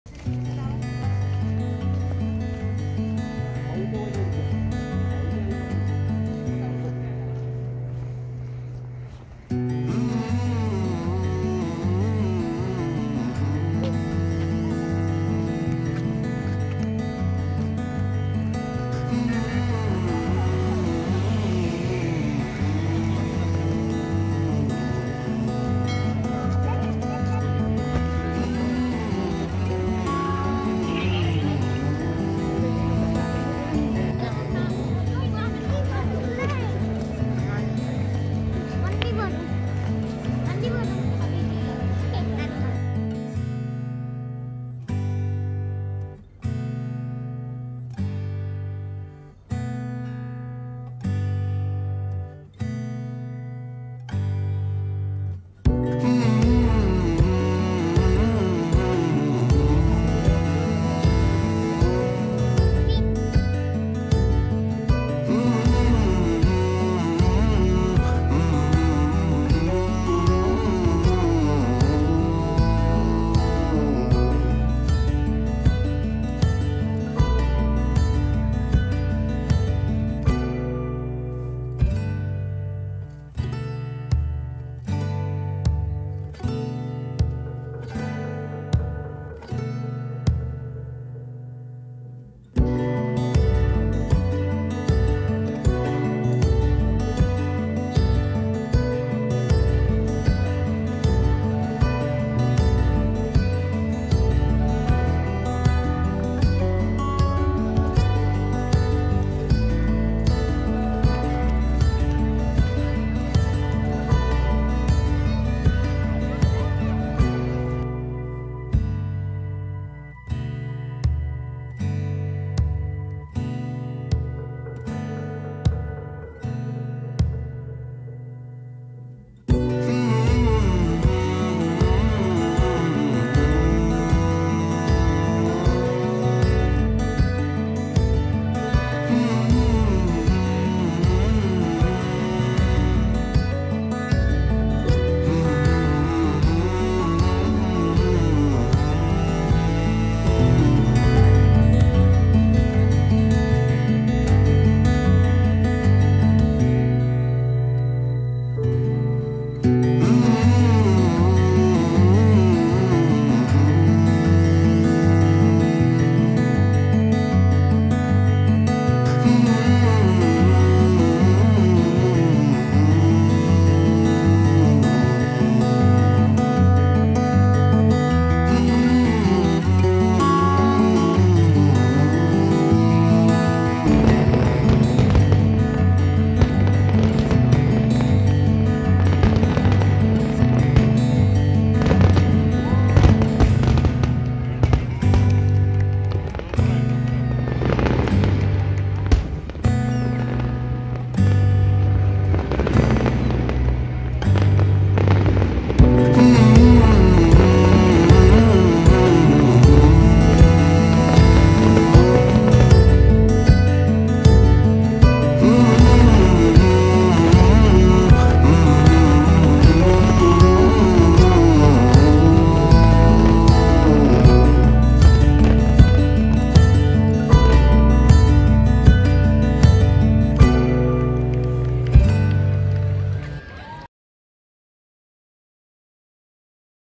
Eiva @ Sheikh Zayed Festival | Al Wathbah - | sheikh zayed heritage festival 2020 fireworks
Hai Friends
I am Eiva from Abudhabi. This is my  channel where you can see some of my activities.